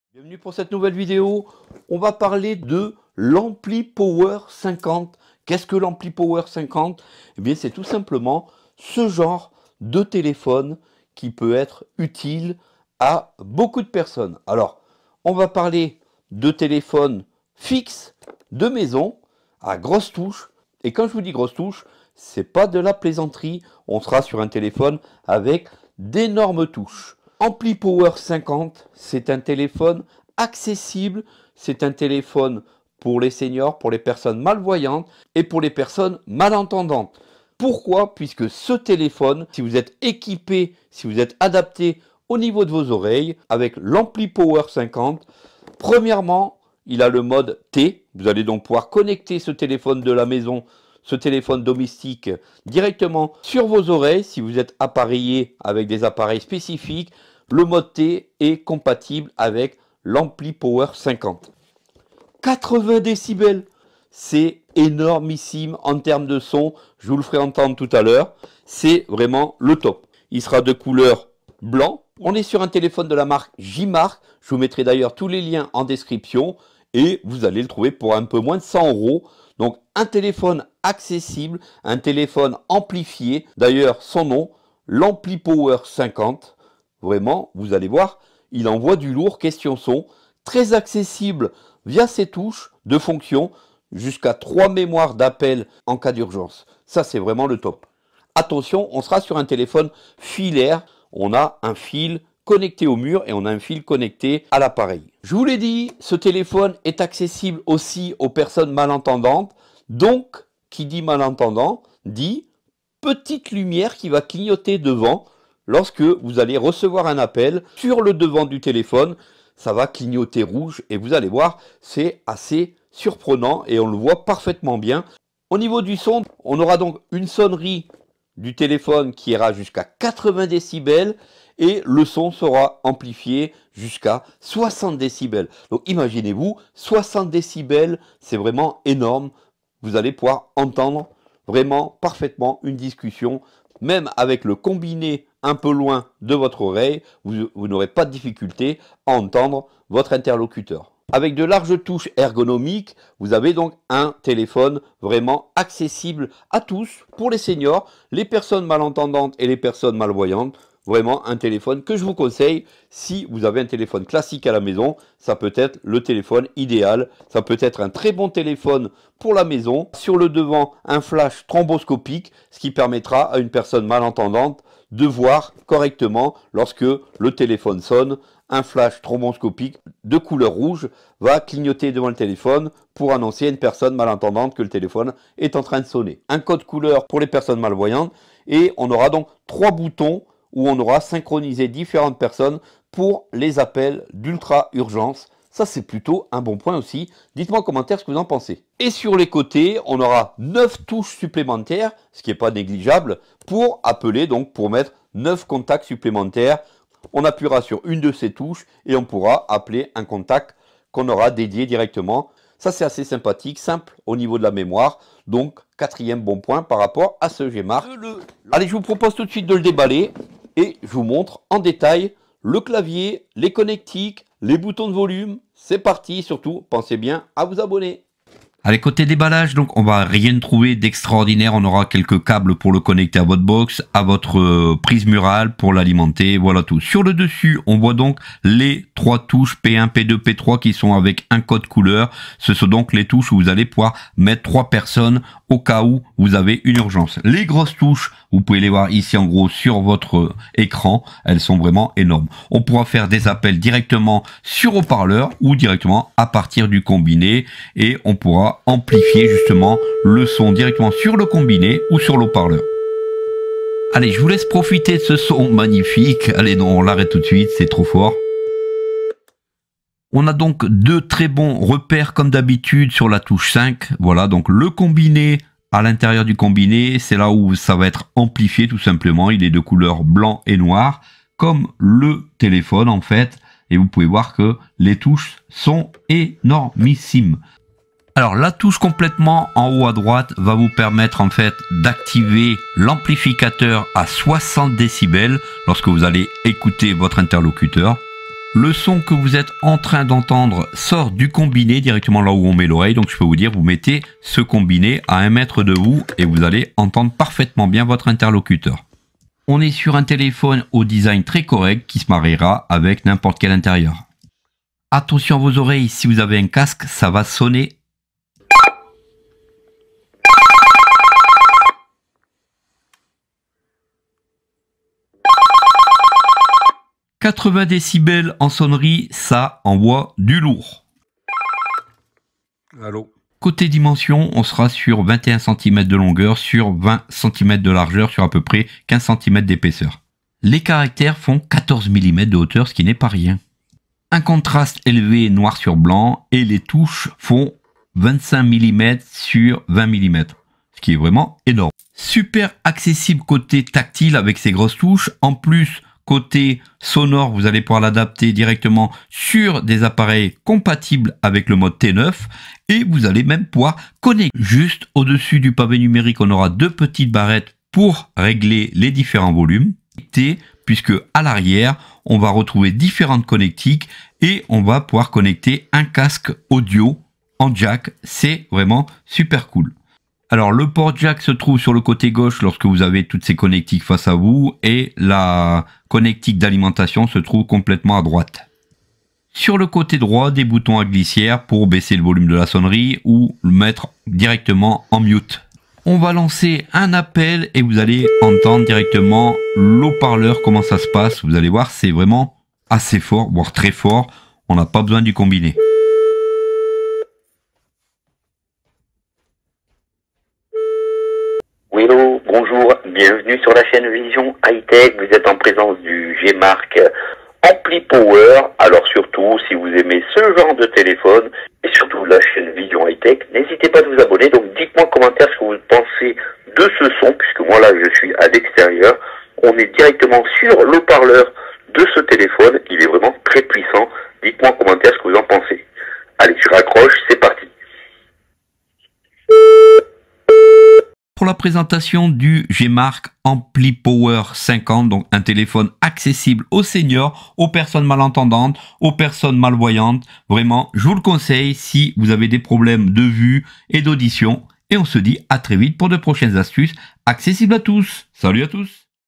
Téléphone Accessible Geemarc AmpliPower 50 : Conçu pour les Malvoyants🦯 et Malentendants 🧏
Le Geemarc AmpliPower 50 est un téléphone amplifié recommandé pour les seniors et les personnes avec des déficiences visuelles ou auditives. Il offre un volume d’écoute très fort, réglable jusqu’à 60 dB, et une sonnerie pouvant atteindre 80 dB. Ergonomique, il dispose de larges touches pour faciliter la composition des numéros. Un flash stroboscopique signale visiblement les appels entrants. Le clavier intègre trois touches mémoires d’urgence et neuf touches supplémentaires pour appeler des contacts enregistrés. Il est compatible avec les aides auditives et dispose d’un mode mains libres .